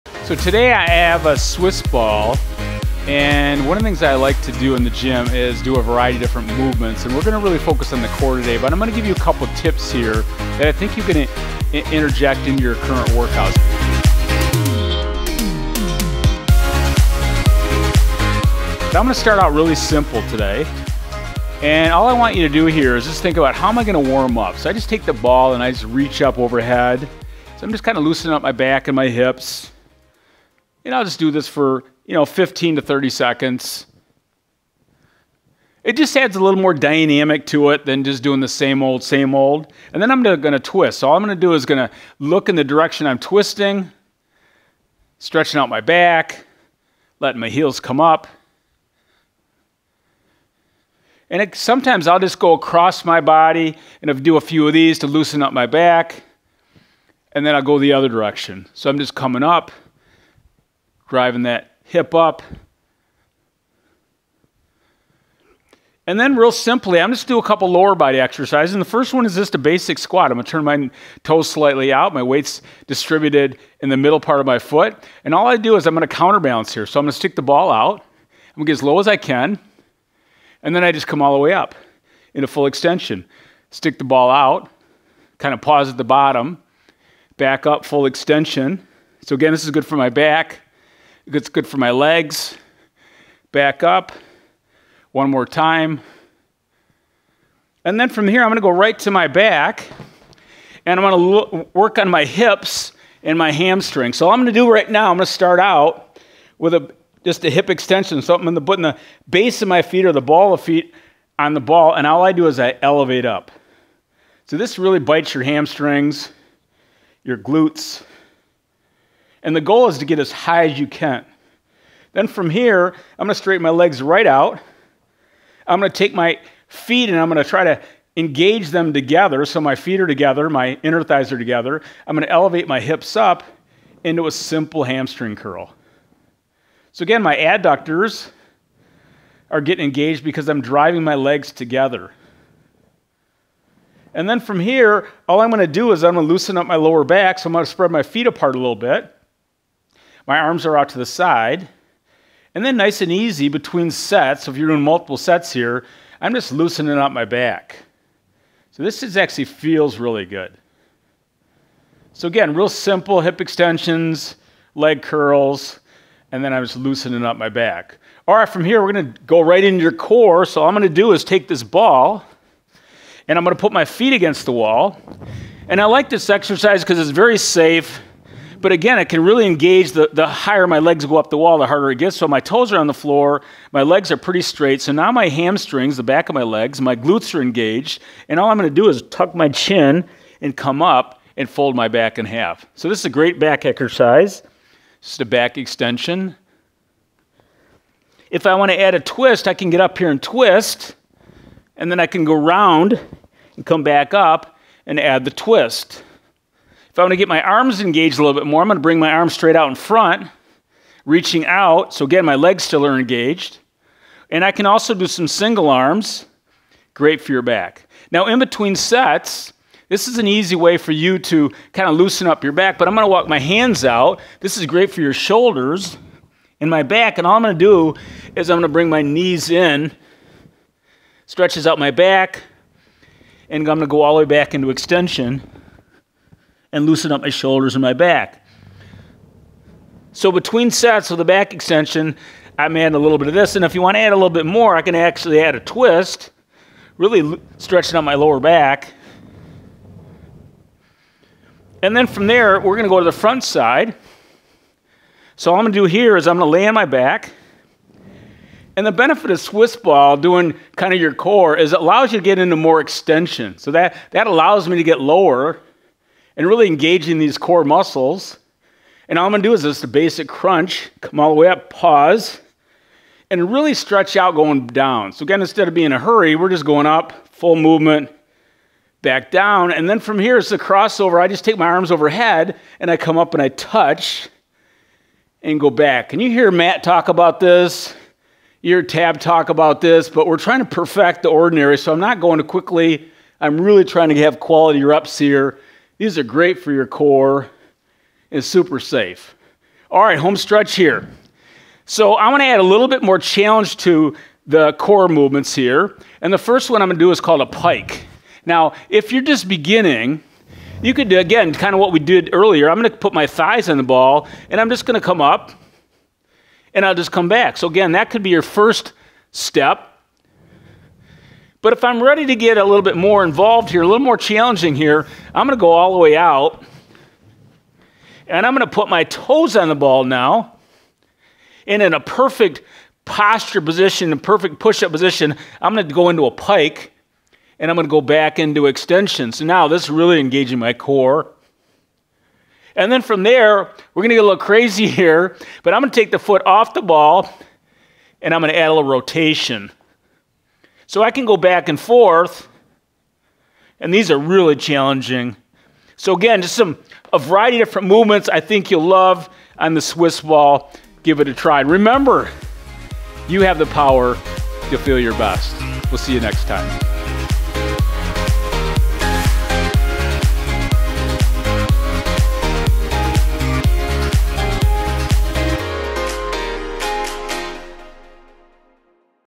Ultimate Swiss Ball Core Workout - Beginner to Advanced
🎥 Ready to take your core strength to the next level? In this *Ultimate Swiss Ball Core Workout*, we’ll guide you from beginner to advanced exercises, ensuring you get the most out of your fitness routine.

One of my favorite things to do in the gym is to mix up my movements, and today we're focusing on the core. We’ll start with some simple warm-ups, like overhead reaches and twists, to get your body loose. Then, we move into lower body exercises like squats, hip extensions, and hamstring curls, which not only target your legs but also engage your back and glutes.

From there, we'll dive into core-specific exercises. Using the Swiss ball, we’ll perform safe yet challenging movements like back extensions, crunches, and the intense pike. Each exercise is designed to push your limits, improve your strength, and enhance your overall wellness.

Remember, the goal here is to challenge yourself and find amazing ways to improve your fitness level. These exercises are not just workouts; they are natural remedies to prevent and aid various health issues, improving the problem at the source.

Give it a try and feel your best! Don’t forget to share your progress in the comments and let us know how these exercises have impacted your wellness journey. 💪

CHAPTERS:
00:00 - Intro
00:20 - Lower Body Exercises
02:48 - Back Exercises
04:11 - Core Exercises